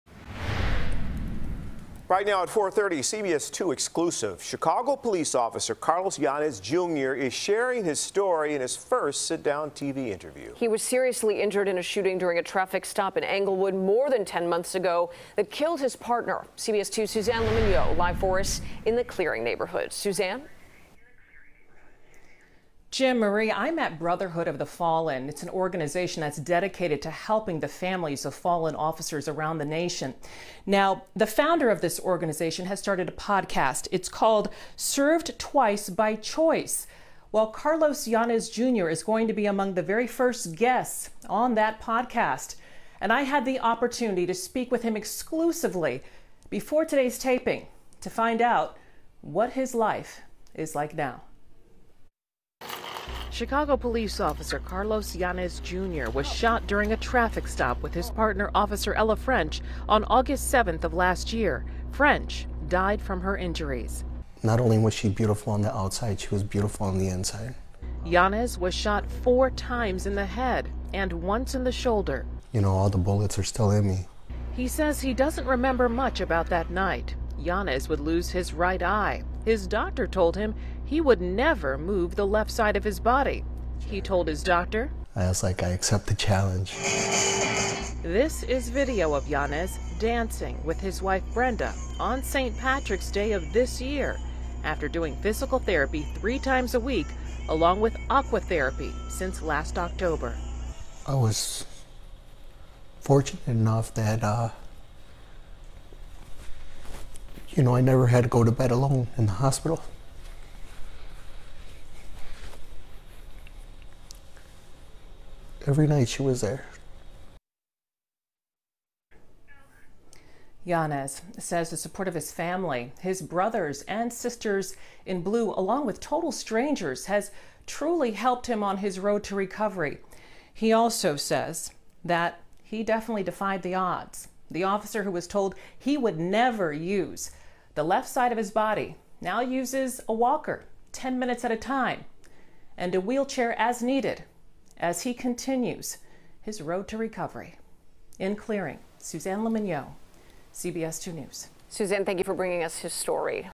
Chicago Police Officer Carlos Yanez Jr. shares his recovery story
Chicago Police Officer Carlos Yanez Jr., who was wounded in a shooting during a traffic stop last year, is sharing his story in his first sit-down TV interview. CBS 2's Suzanne Le Mignot reports.